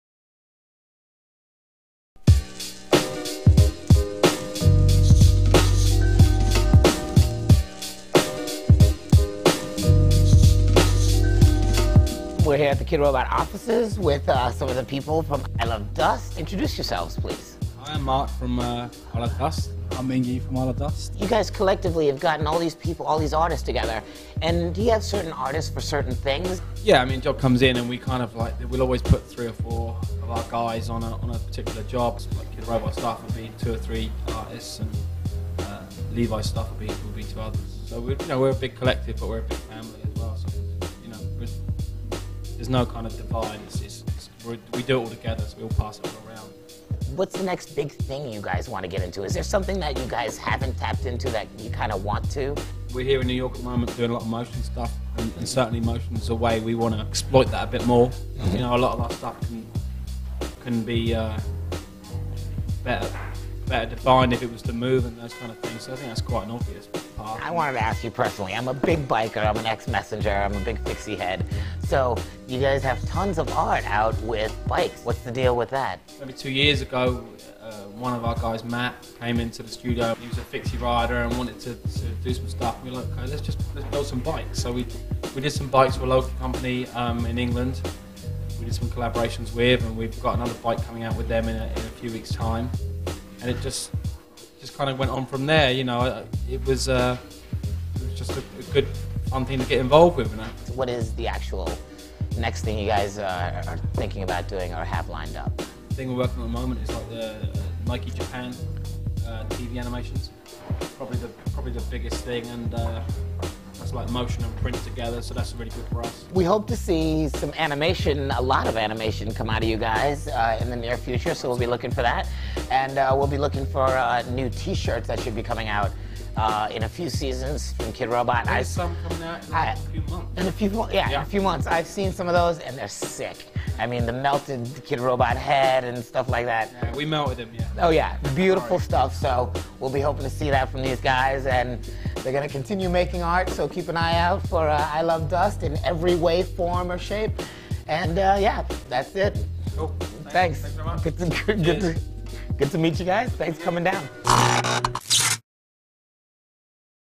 Artist Interview - ilovedust!
Design collective, ilovedust talks art, design and bikes with Kidrobot.  Distributed by Tubemogul.